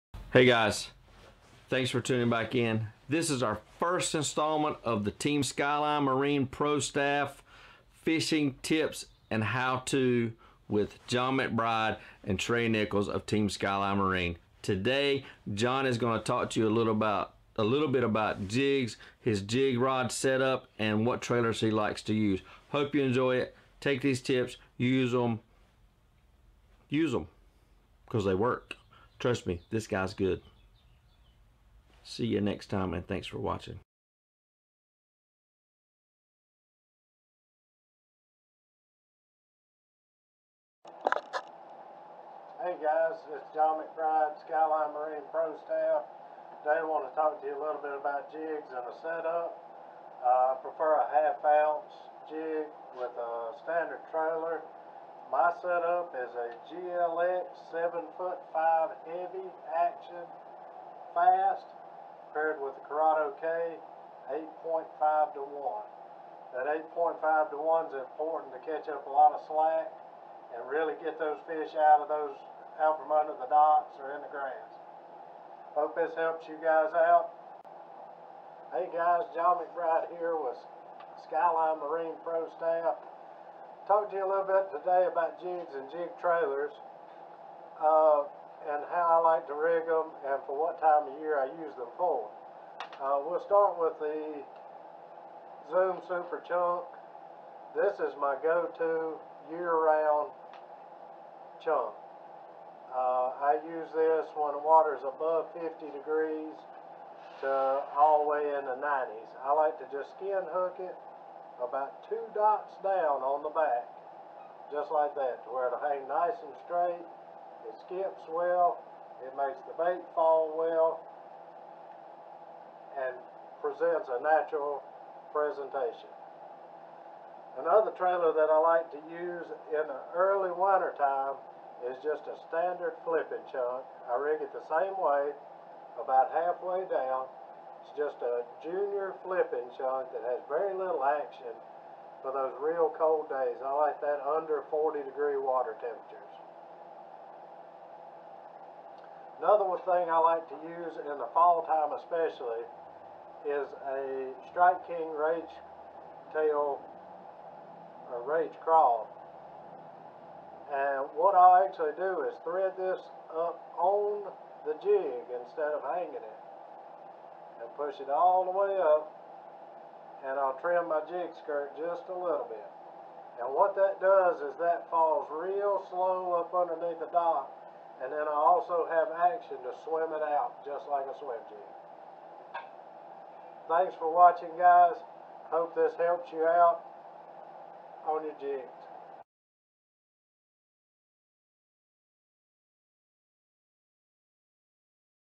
Jigs 101 With John McBride
JIgs 101 With John McBride : Skyline Marine pro staffer John McBride briefly  explains a little bit about Jigs , his rod and reel set up and the trailers he likes to use.

VIDEO CAMERA

VIDEO MIC

VIDEO LIGHT

DSLR CAMERA

ACTION CAM

SD CARD

SD CARD FOR ACTION CAM